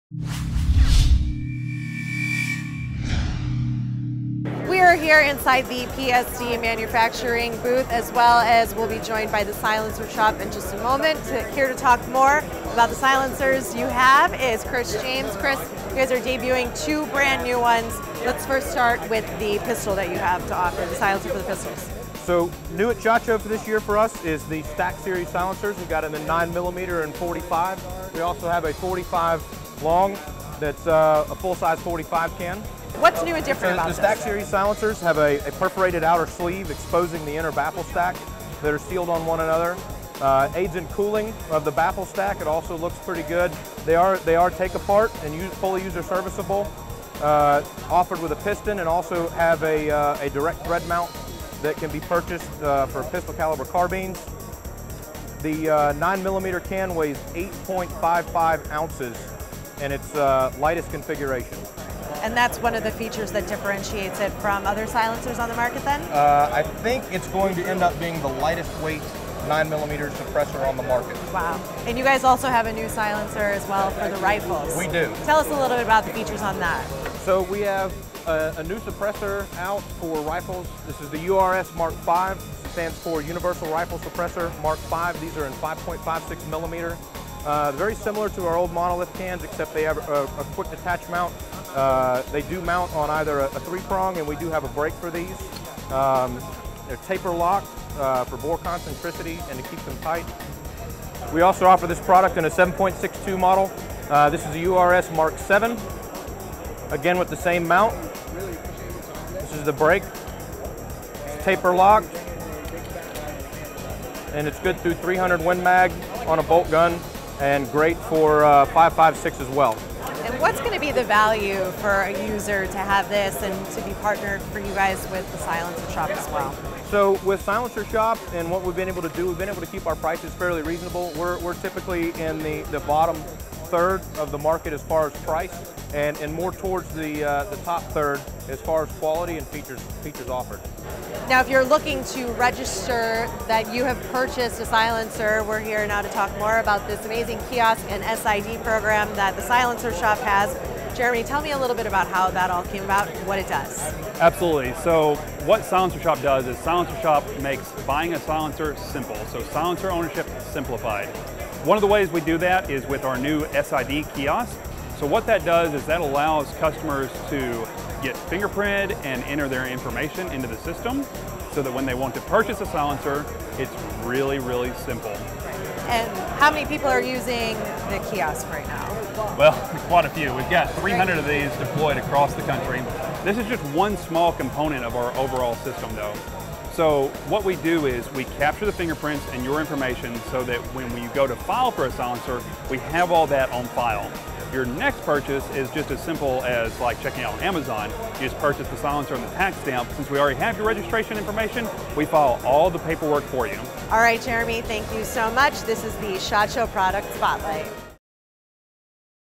SHOT Show Product Spotlight: PSD | 2017 SHOT Show
SHOT Show Product Spotlight: Rachel Kopczyk visits the PSD Manufacturing booth, #4161. PSD Manufacturing is debuting 2 new silencers. Rachel also speaks with The Silencer Shop about their new kiosks and SID program which makes buying a silencer simple. For more, be sure to visit the PSD Manufacturing